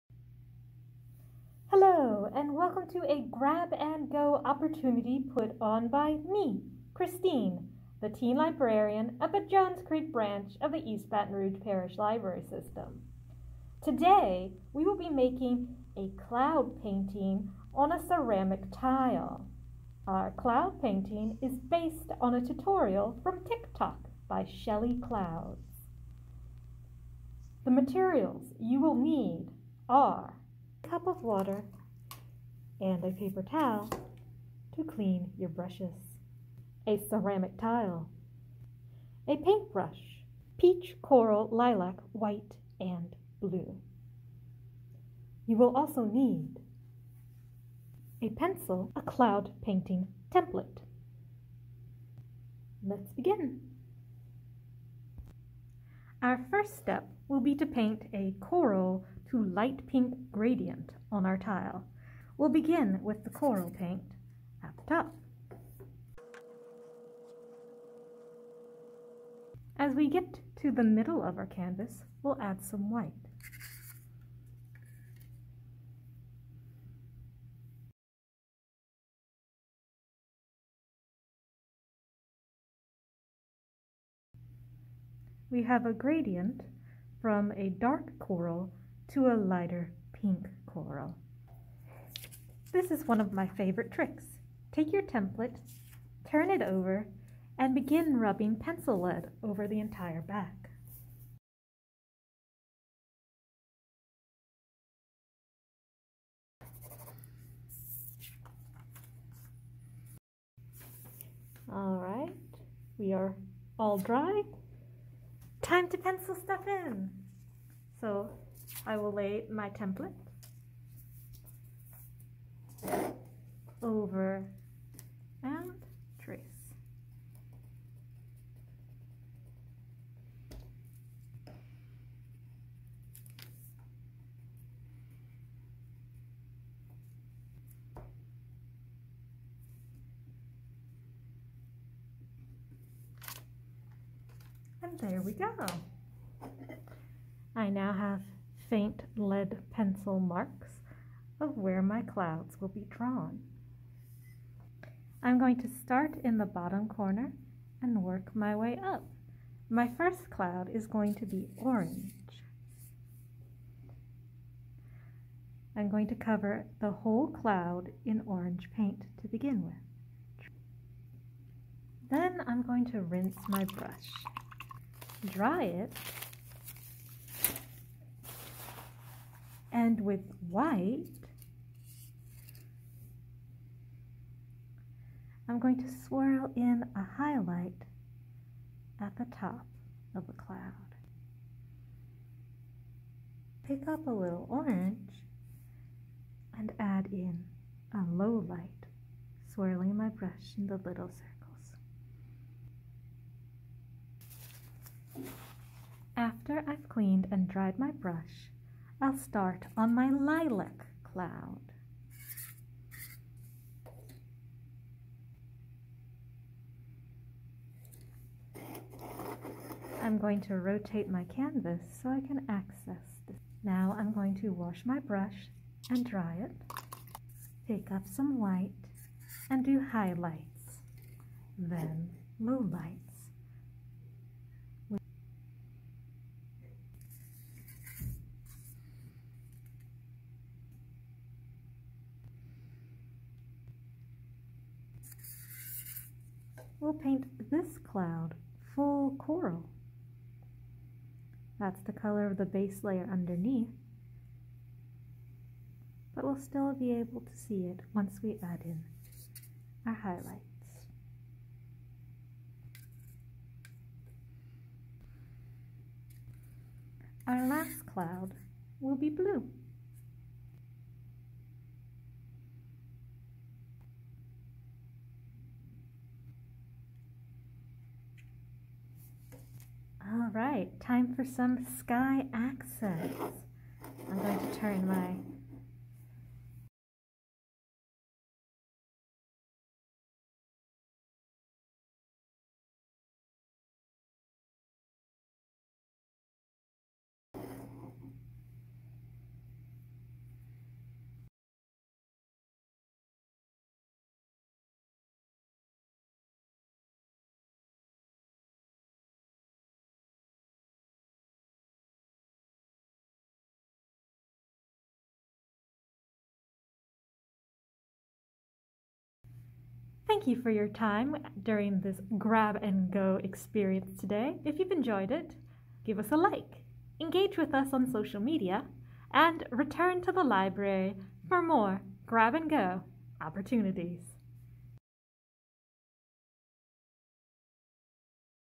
Ceramic Tile Cloud Paint | Grab & Geaux
Learn to paint a starlit scene of fluffy clouds on a ceramic tile.